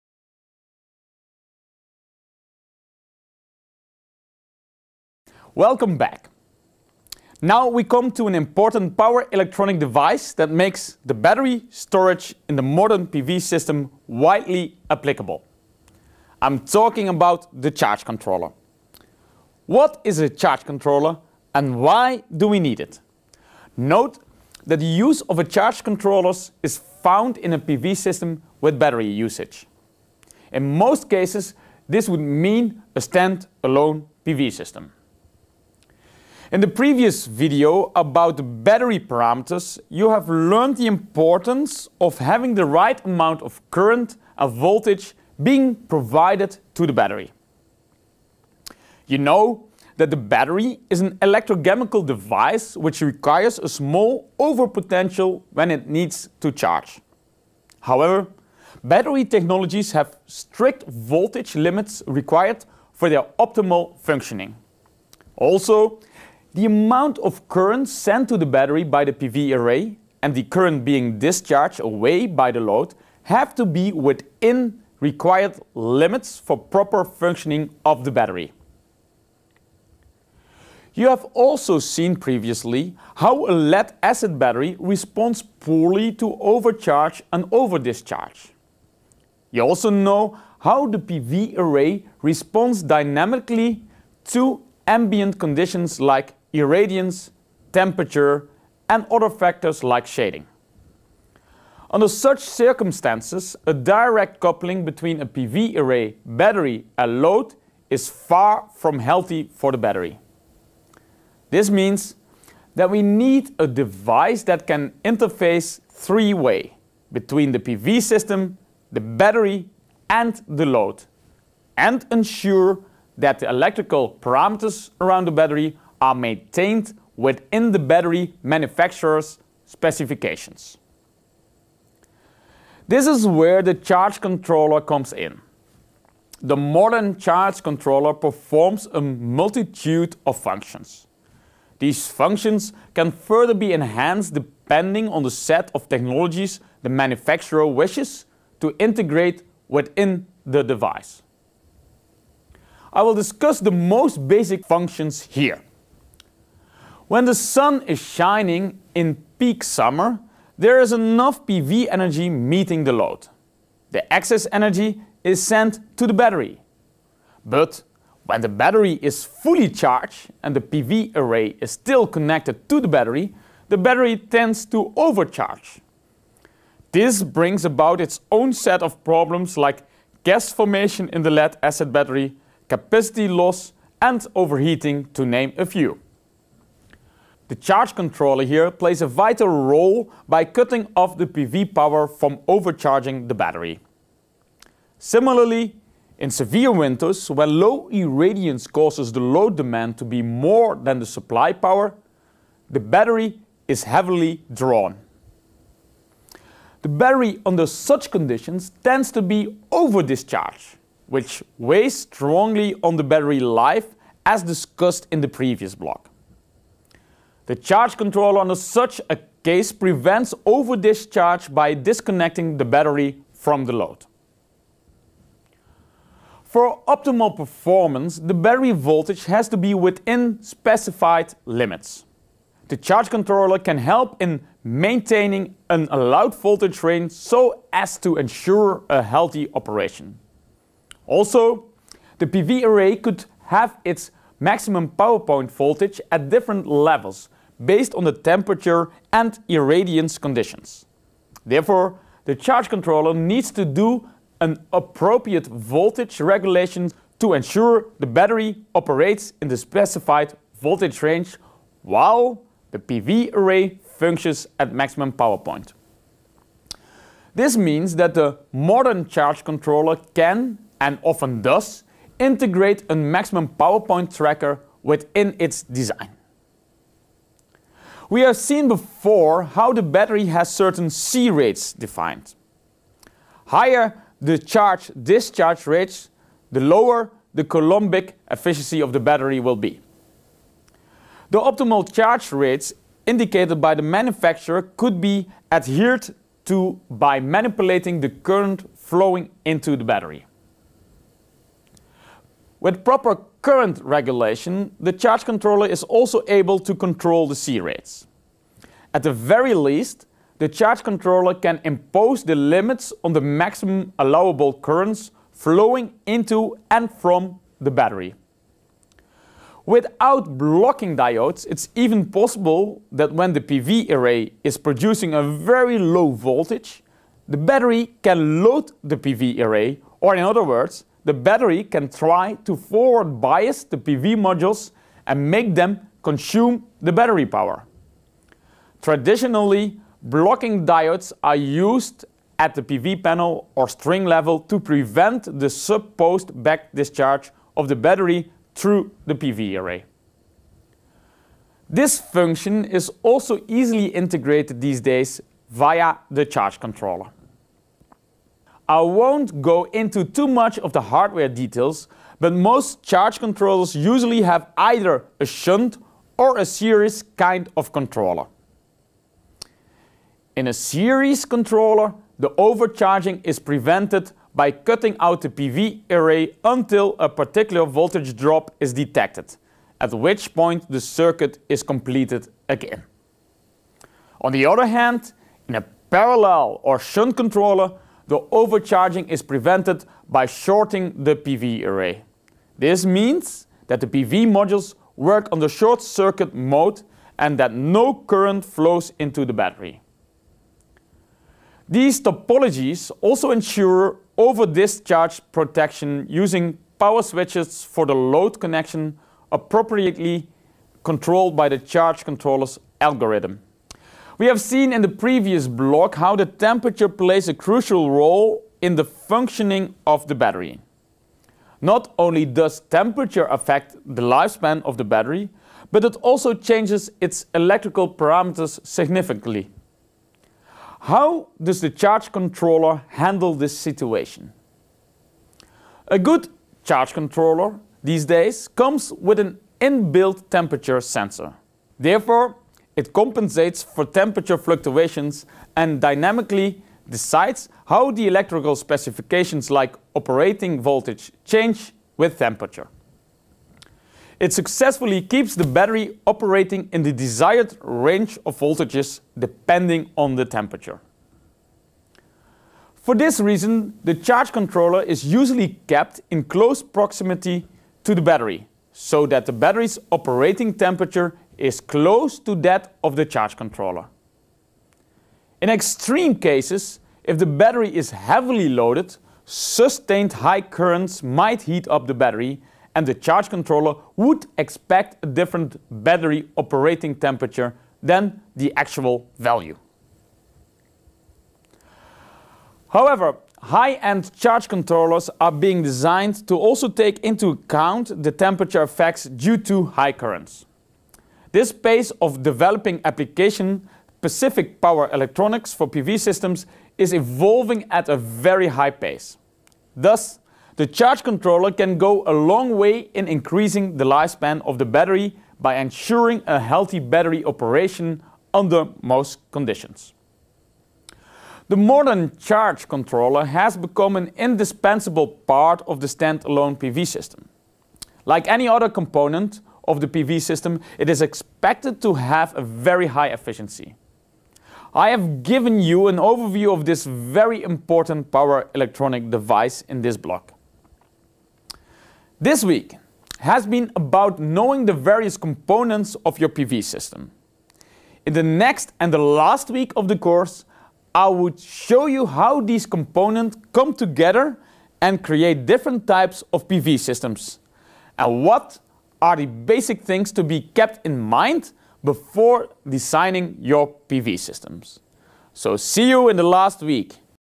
Learn Solar Energy | Change Controllers
In this video, you'll learn about an important power electronic device that makes the battery storage in the modern PV system widely applicable: the charge controller. Take the full course, test yourself, and earn a certificate on edX